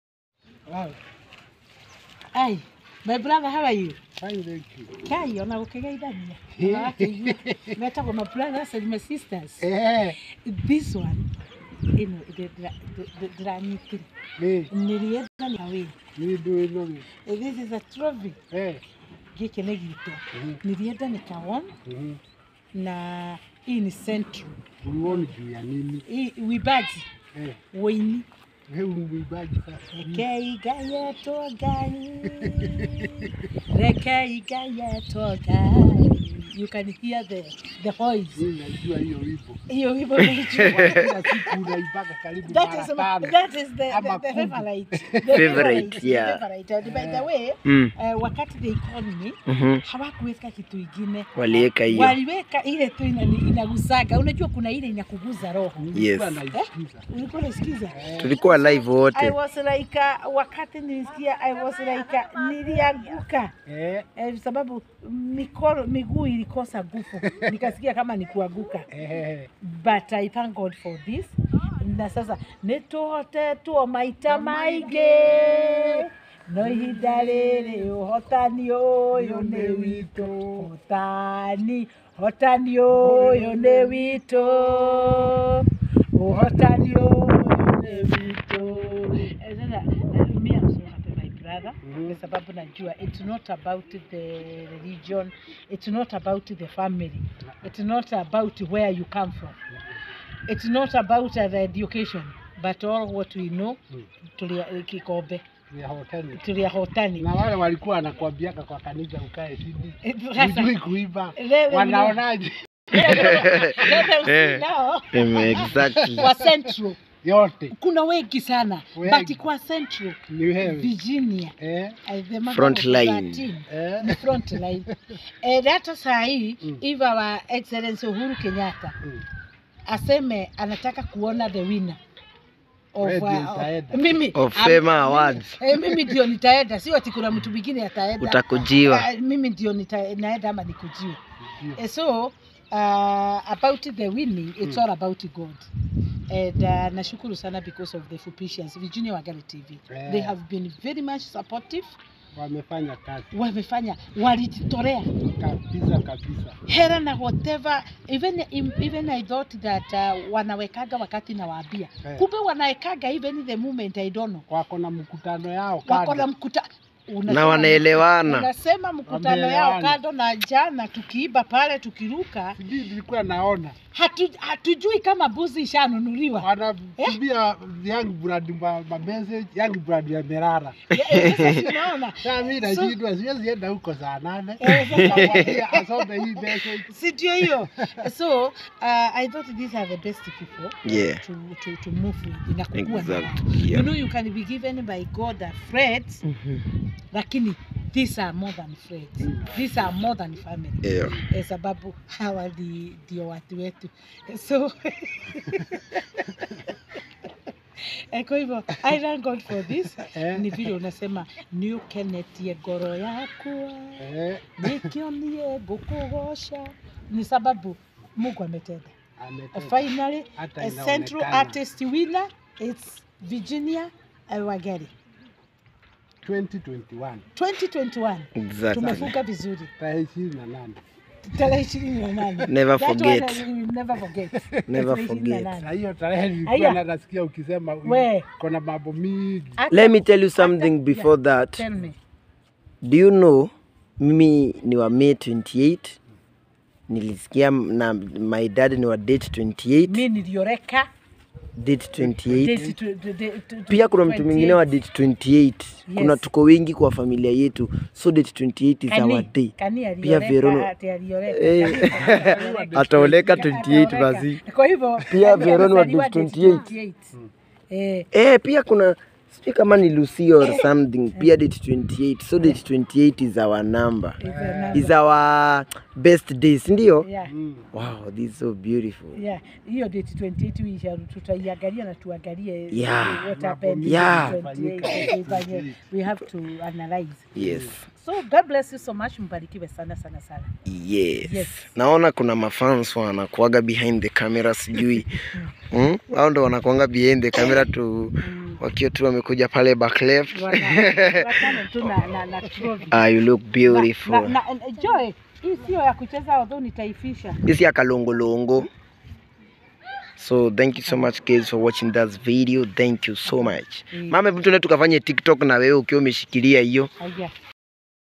VIRGINIA'S BROTHER PATRICK THUITA SO HAPPY AFTER MOM VIRGINIA SHOWED HER THE AWARD SHE WON..
Virginia wangari tv changing lives